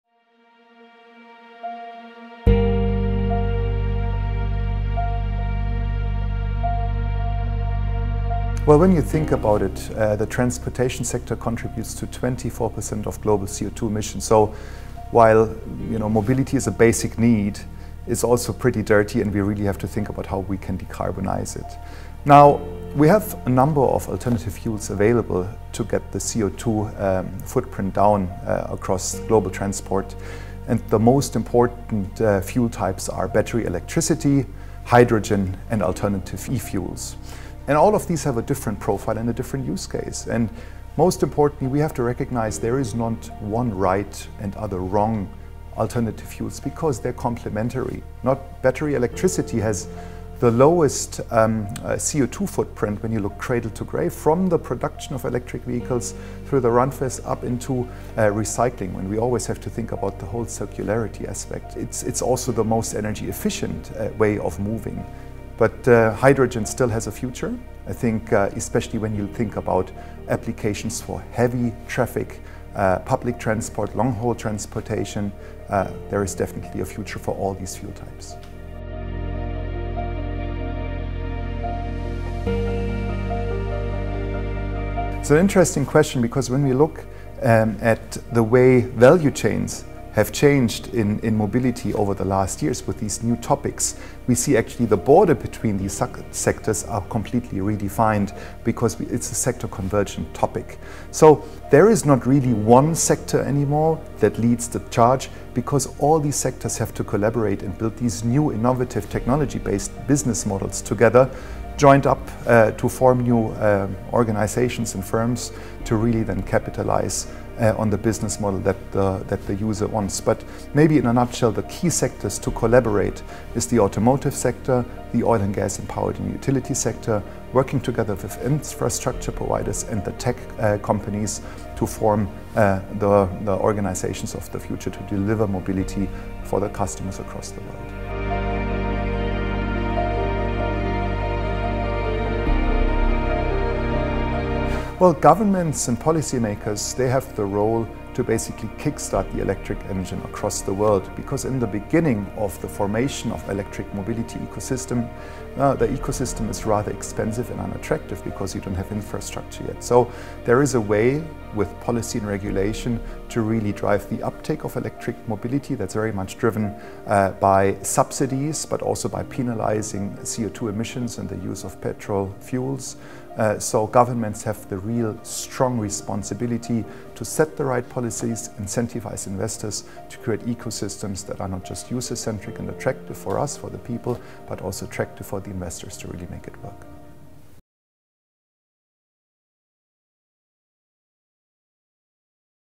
PwC insights on the sustainable transportation sector
Heiko Seitz (PwC eMobility Leader) on what it will take to deliver a more sustainable transportation sector.

Topics include:
- Heiko’s vision for the blend of alternative fuels in achieving a more sustainable transportation sector
- The sub-sectors that are leading the charge in sustainable transportation
- The role of government in achieving a decarbonised transportation sector